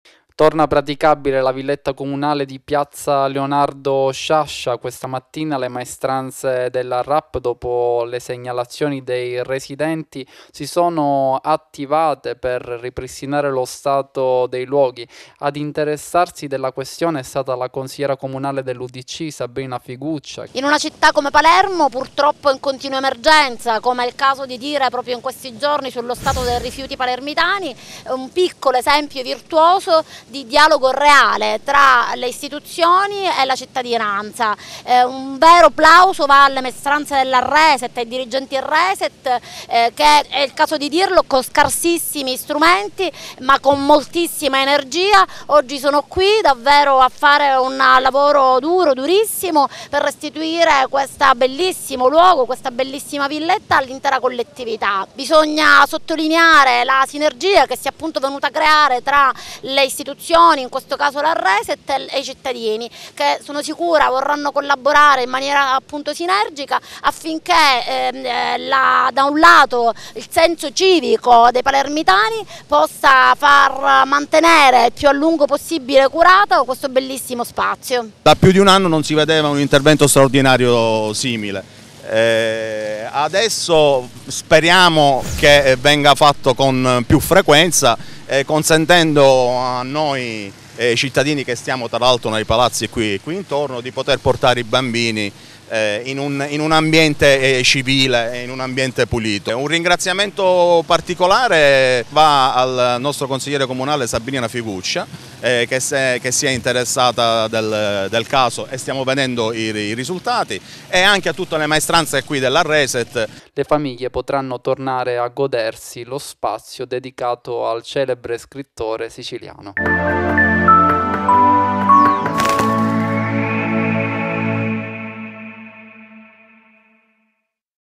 Villa Comunale di Piazza Leonardo Sciascia restituita alla fruizione della cittadinanza
“Da tempo la villetta comunale di piazza Leonardo Sciascia era abbandonata all'incuria e al degrado. Dopo numerose segnalazioni di  cittadini e residenti, abbiamo chiesto l'intervento urgente degli operatori  Reset che, ieri, sono intervenuti puntualmente eliminando rifiuti  e tagliando le sterpaglie, restituendo decoro alle aiuole.”  – Ad affermarlo è Sabrina Figuccia consigliere Udc del comune di Palermo -.
“Lo stato di abbandono in cui versava la villa era il motivo principale di preclusione per la fruizione di uno spazio verde pubblico che, da oggi potrà essere riconsegnato alla collettività.”  – Prosegue il consigliere –.
“Un plauso agli operatori Reset che malgrado un’inadeguata dotazione di strumentazione tecnica, con grande volontà sono riusciti a rendere vivibile uno spazio verde che il senso civico dei palermitani dovrà custodire” – Conclude Sabrina Figuccia -